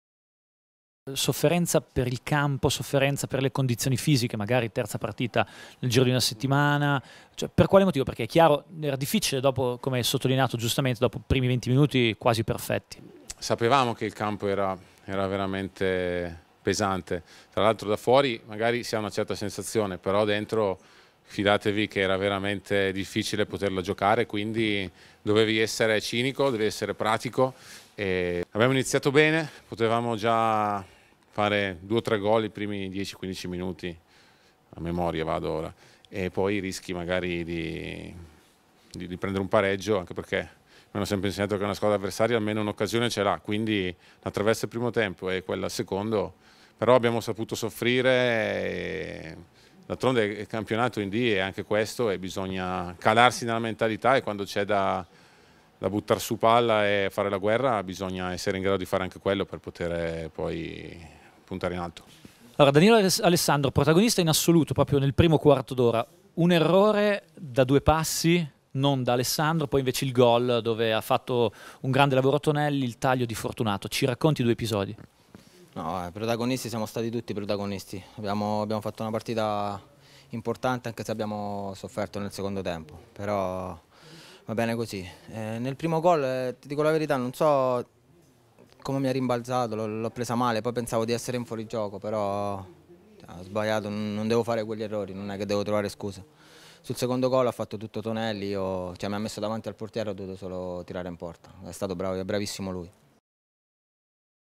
CALCIO: Agliardi si mette l'elmetto "Dobbiamo calarci nel fare la guerra" | VIDEO
Se da una parte il Cesena a Castelfidardo è stato sprecone in termini di occasioni create, dall’altra la difesa ha confermato di essere la migliore del torneo non incassando reti. Per Federico Agliardi e Danilo Alessandro non c’è però da abbassare la guardia.